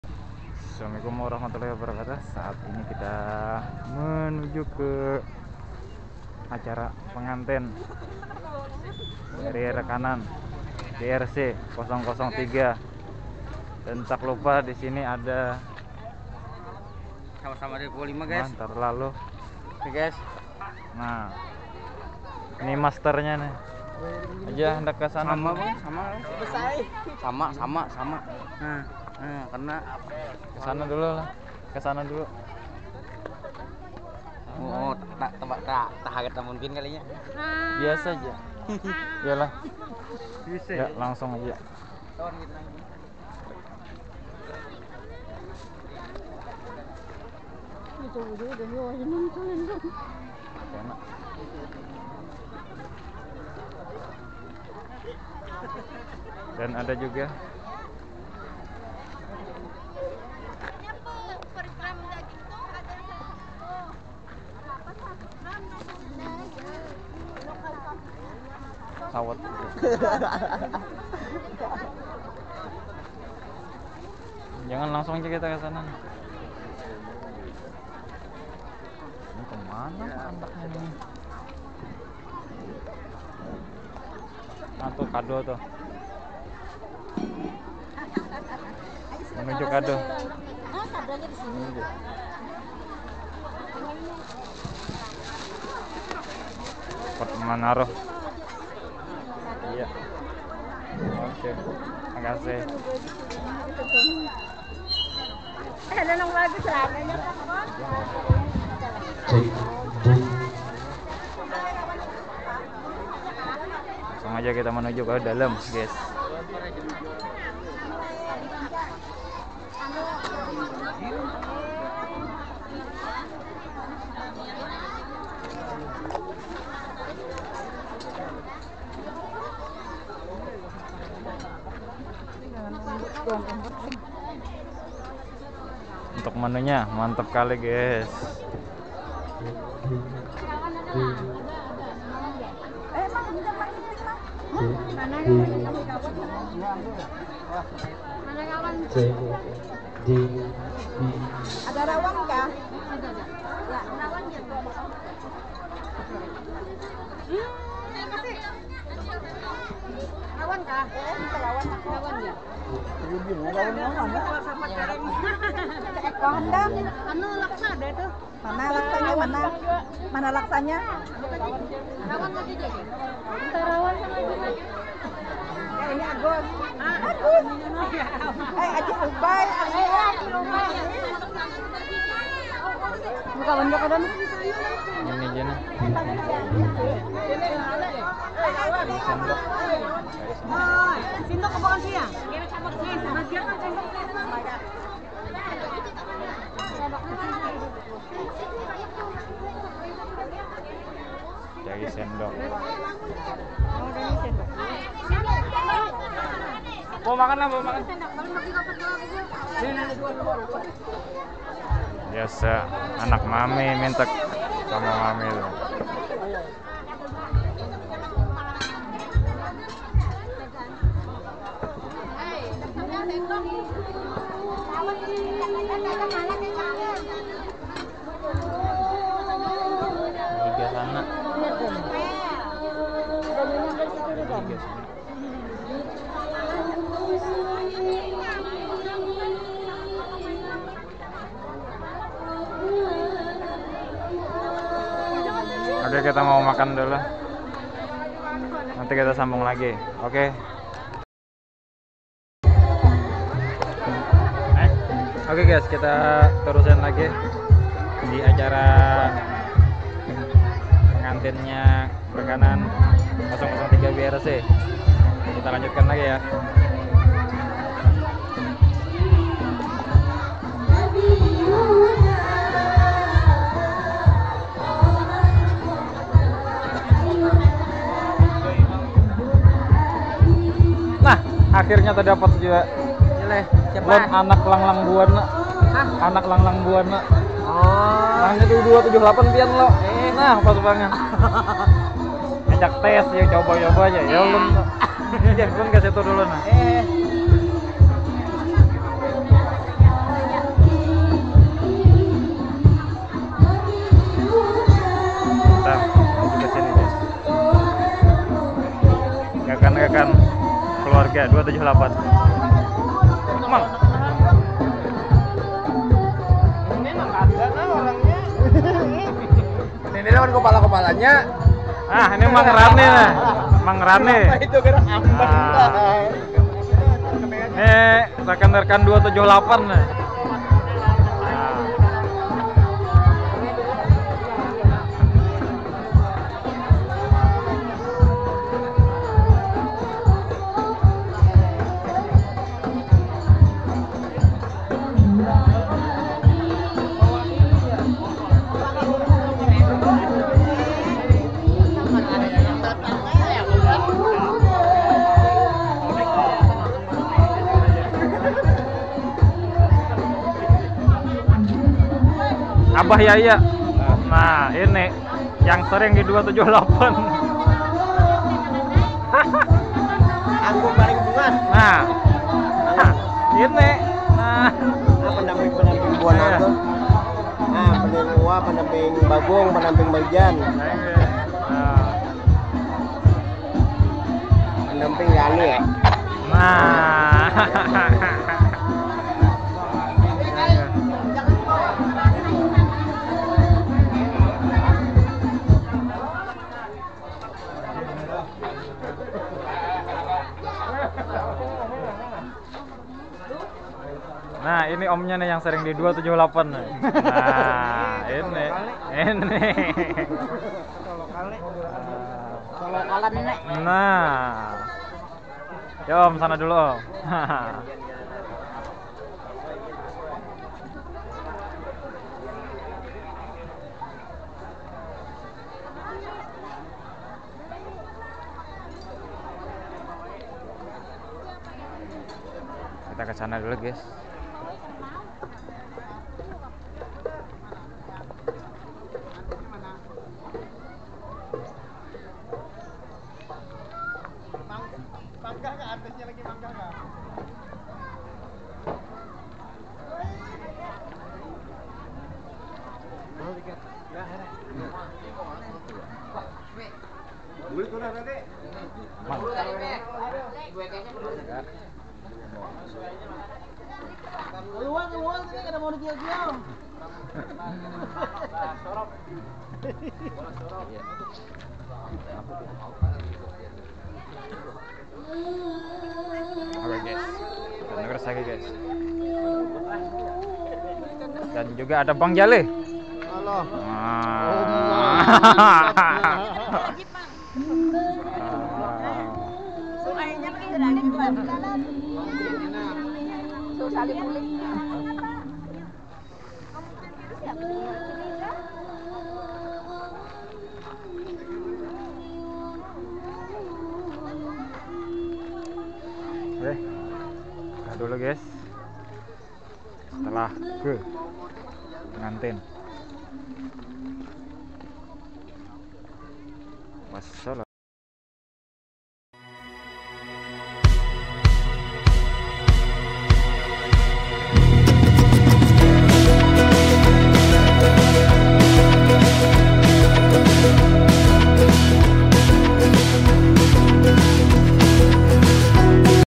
vlog spesial ke undangan rekanan BRC emergency
hai gaes..
vlog kali ini,, aku bikin pas mau ke acara resepsi pengantin rekanan, next, simak sampai habis. 🆗